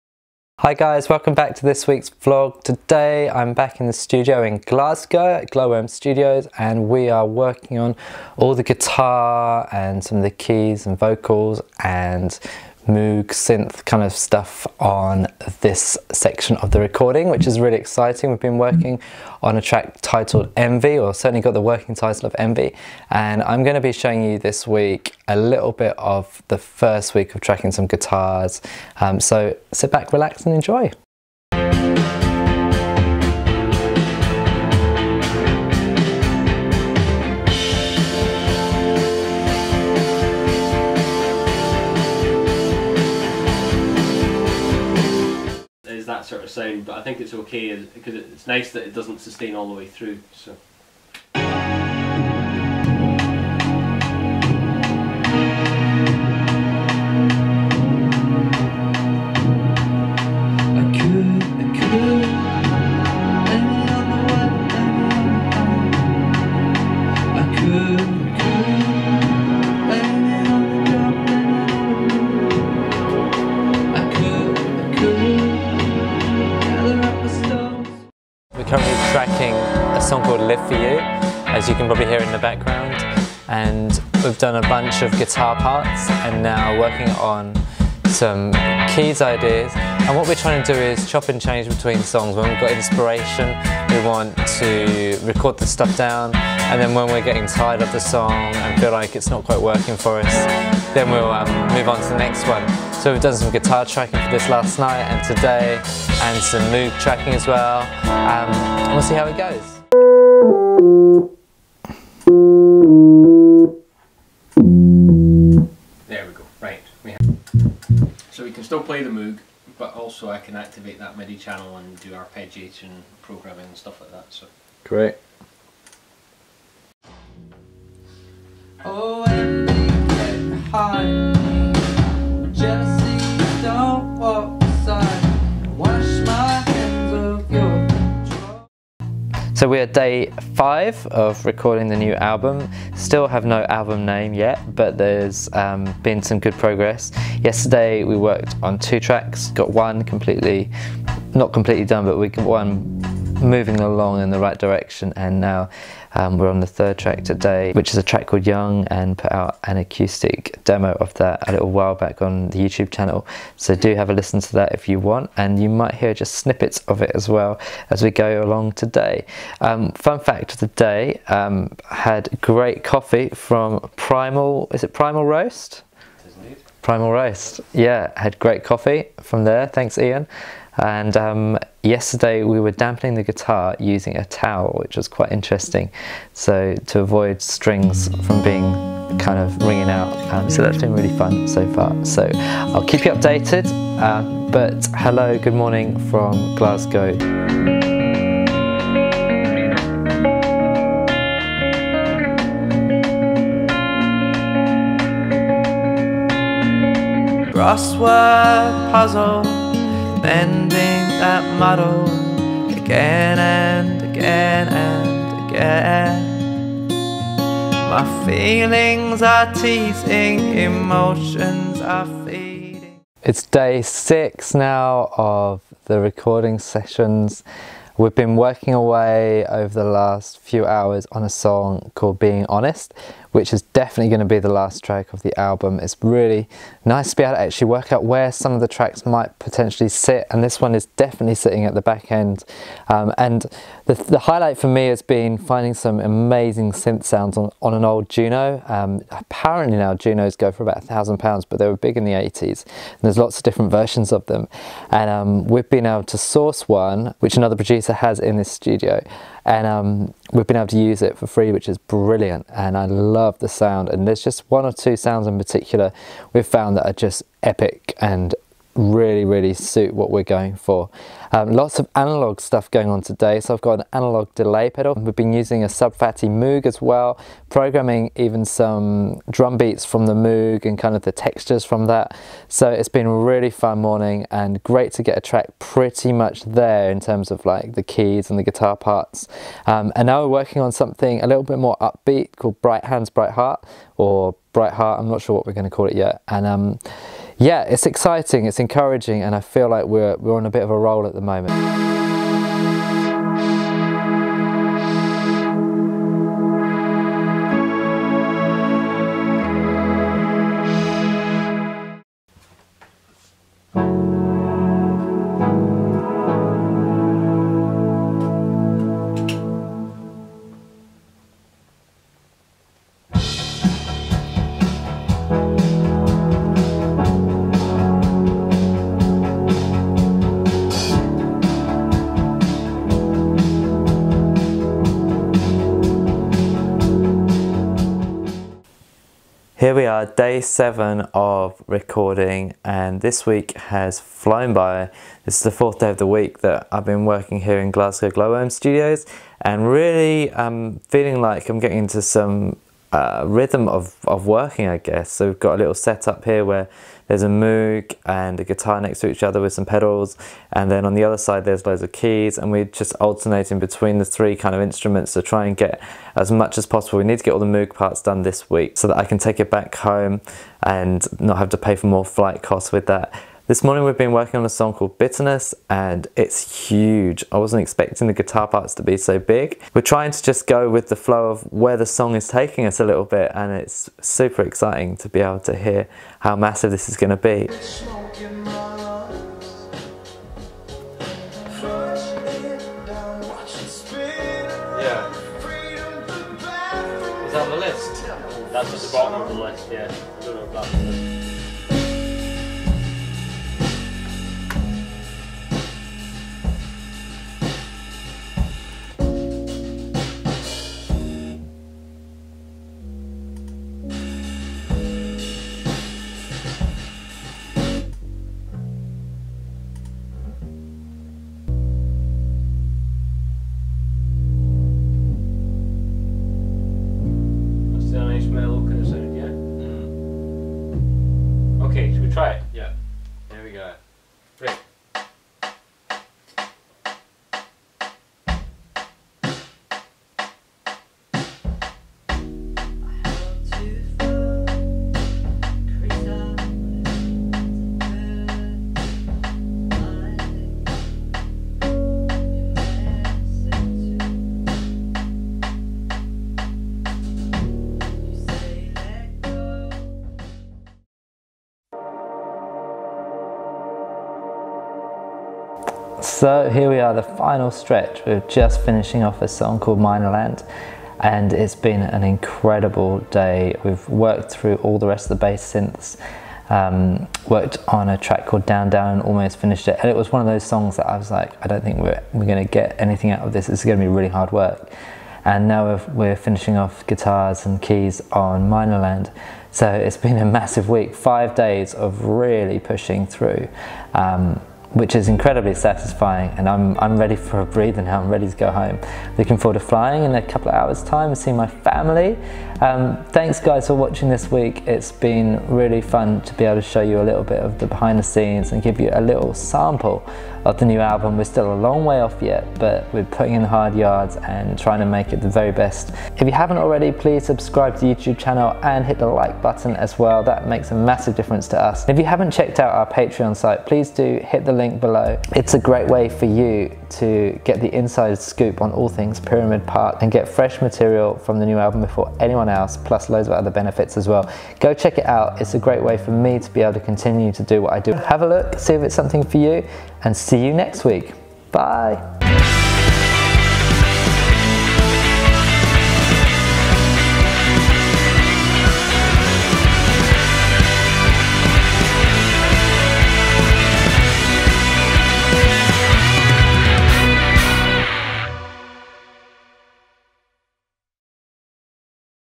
Pyramid Park - New Album Recording Session 2
Get THREE FREE Indie Pop songs from Pyramid Park.  I’ll send you three of my favourites.  to download the first.

This week I take you behind the scenes in my new album recording sessions. This is your first taste of the new album tracks...enjoy!